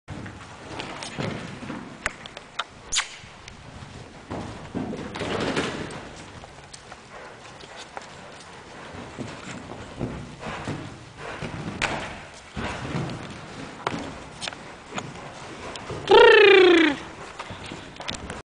FLANKIER
wał.ur.2003, m.Falanga wlkp./Illit,o.Dżamir xx/Babant, świetny ruch, jezdny, finalista MPMK 2007 w wkkw.